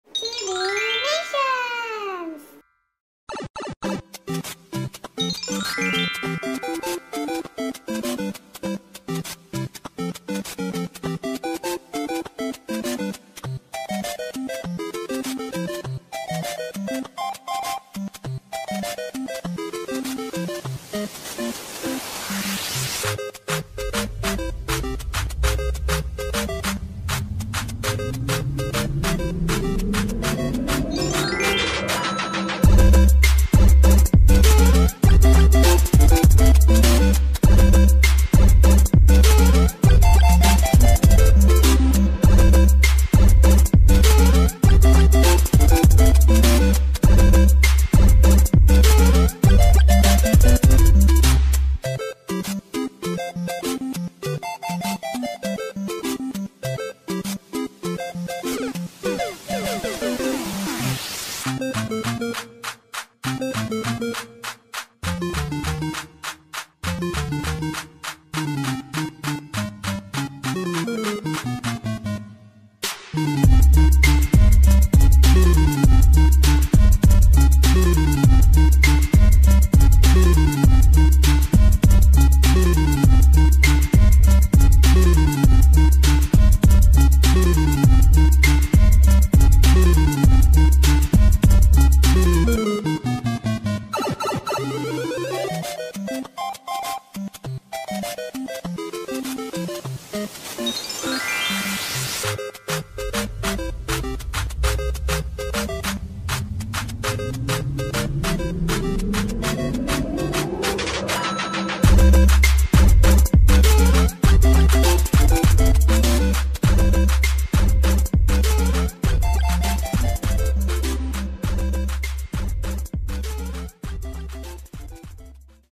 Menggambar dan Mewarnai Super Mario Bros | How to Draw Super Mario Bros | CHIBINATIONS®
Halo teman-teman!!

Kali ini kita akan menggambar dan mewarnai Super Mario!
Yang suka ayo ikut menggambar bersama!

Selamat menonton!